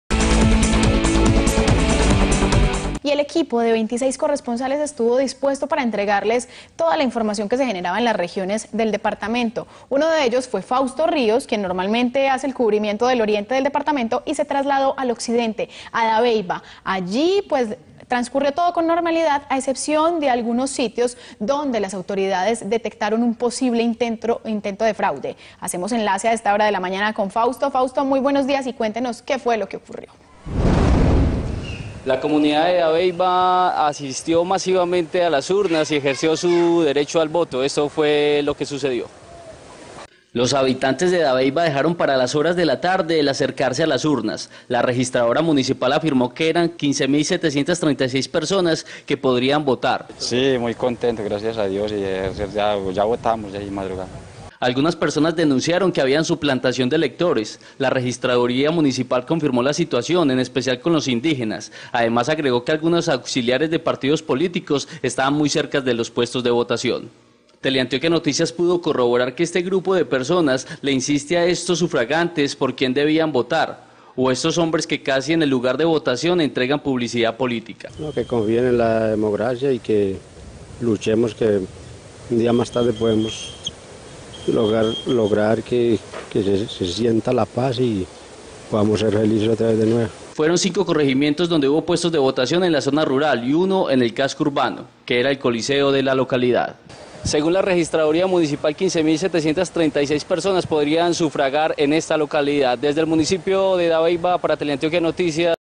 En Dabeiba fueron detectados intentos de fraude
Aunque en General las elecciones en el municipio de Dabeiba se registraron sin contratiempos, sí existieron algunos puntos donde las autoridades detectaron algunos intentos de fraude.